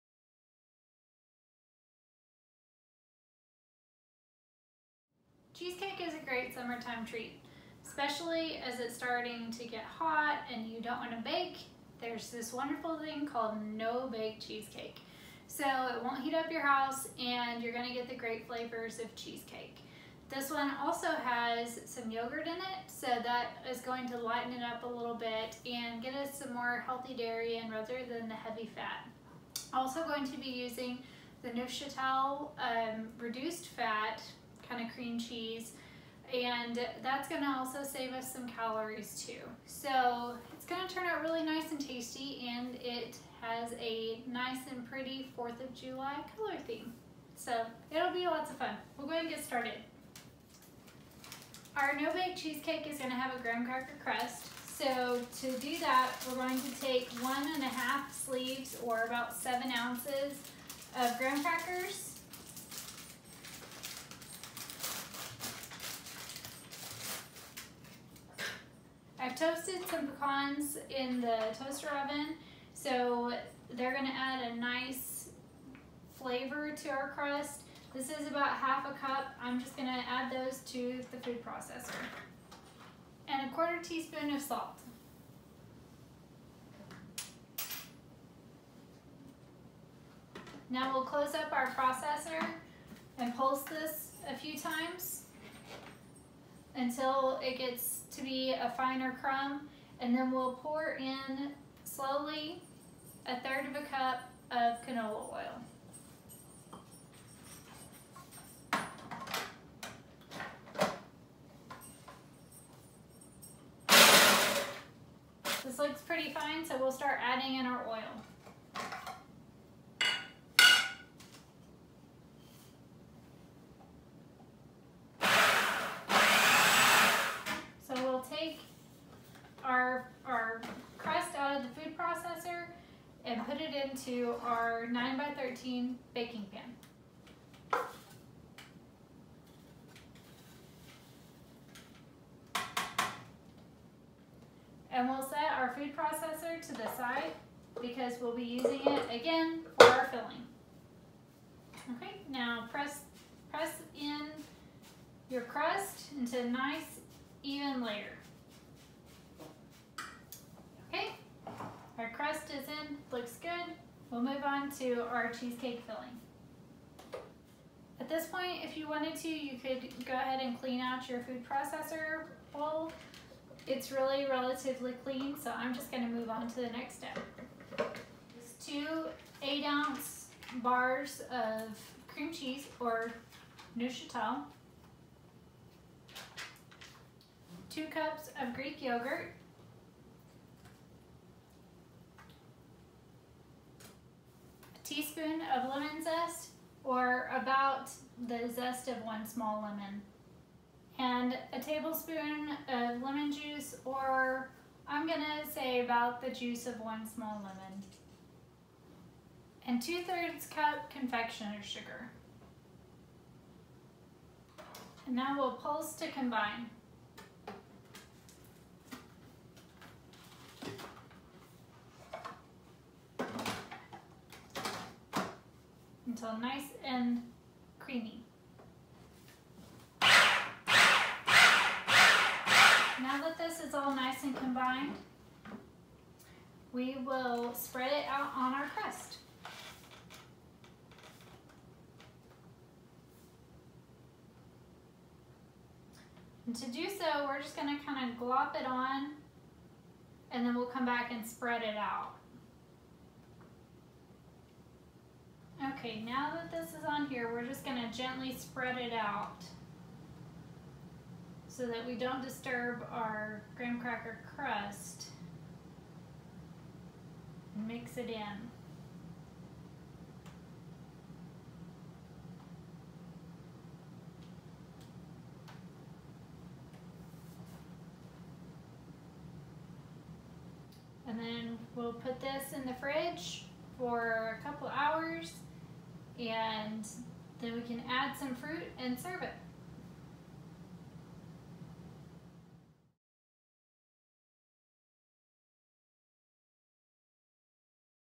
Red, White, and Blue No-Bake Cheesecake
This dessert is the perfect addition to celebrating the 4th of July with a backyard barbecue! It comes together quickly and is a cool treat on a hot day. Here’s the recipe